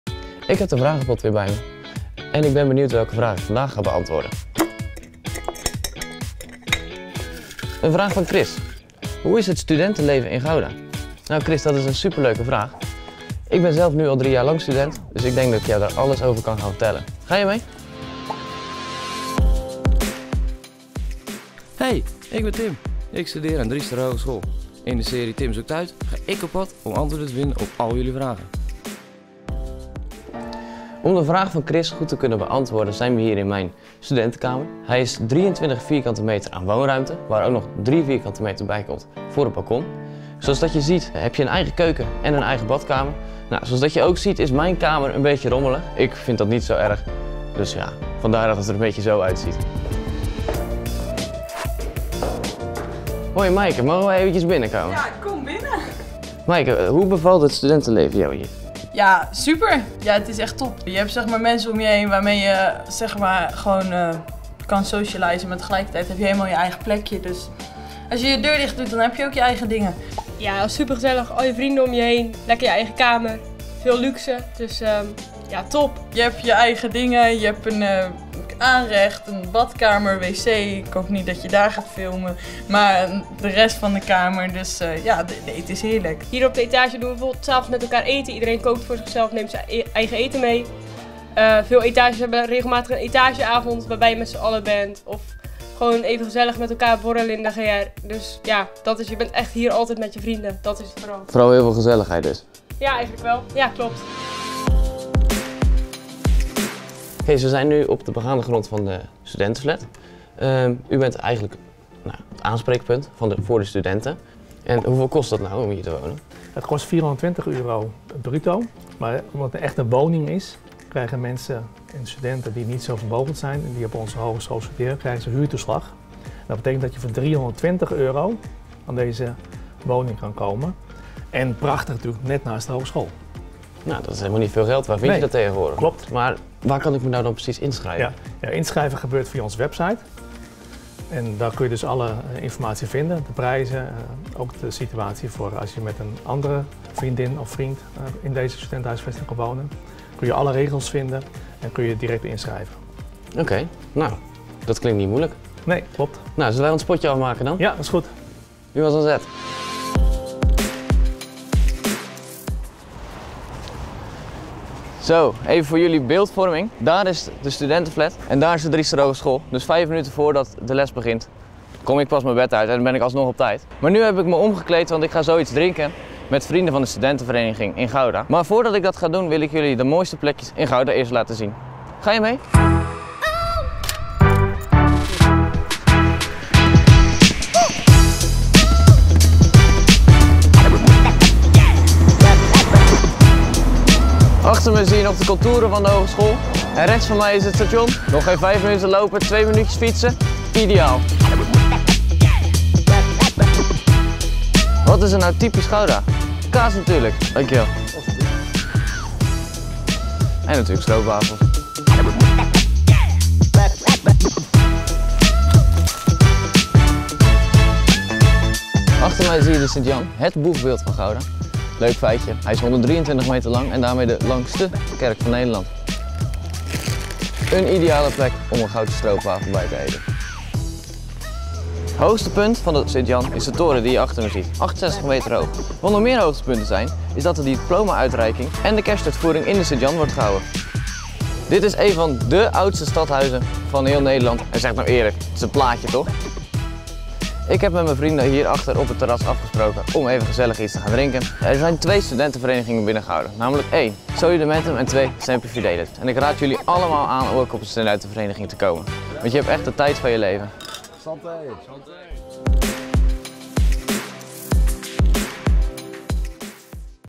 TIM ZOEKT UIT #3: Hoe is het studentenleven in Gouda?
Zit je op de middelbare school en weet je nog niet wat je wil gaan studeren? In deze vlogserie gaat pabostudent Tim van Driestar hogeschool met een pot vol vragen van studiekiezers op pad. Hij zoekt naar antwoorden op vragen over het onderwijs, de pabo en studeren in Gouda. In deze aflevering de derde vraag: Hoe is het studentenleven in Gouda?

Elke donderdag om 15.00 uur een nieuwe video!

I N S T A G R A M: @driestarhogeschool